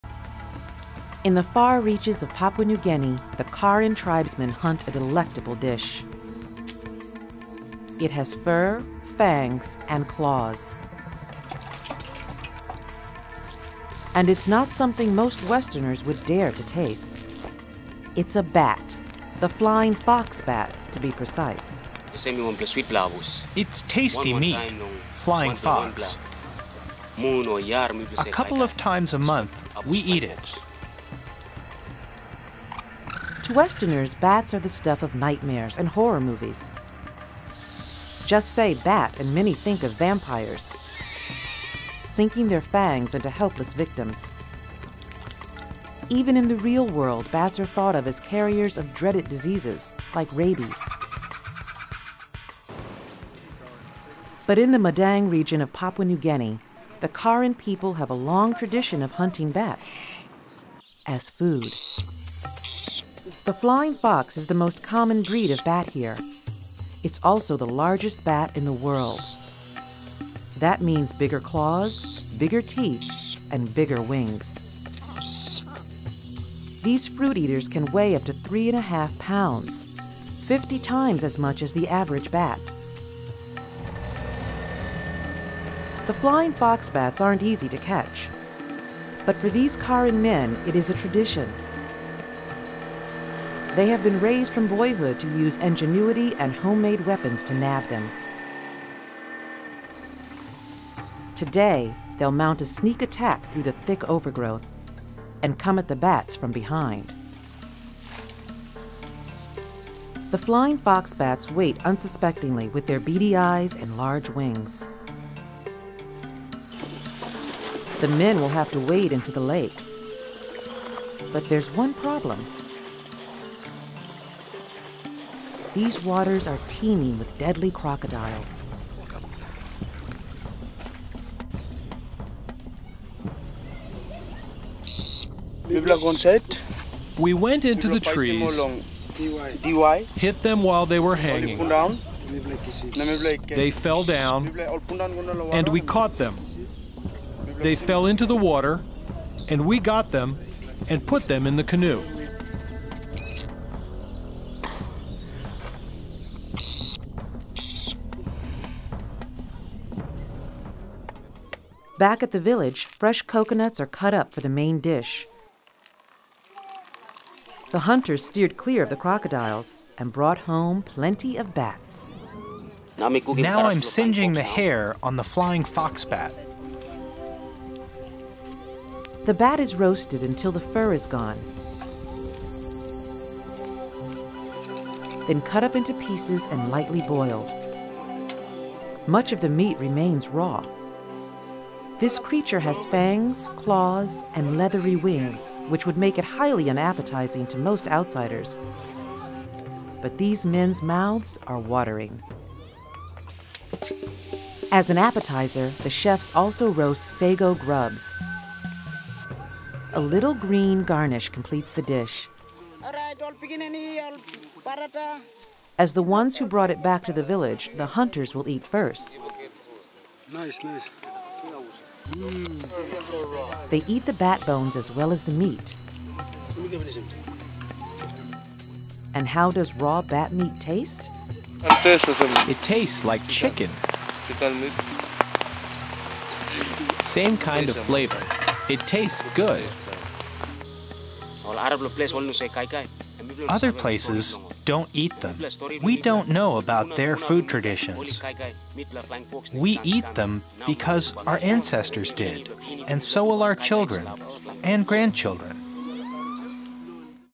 Eating Bats | National Geographic
In the far reaches of papua New Guinea, the Karan tribesmen hunt a delectable dish - with fur, fangs and claws.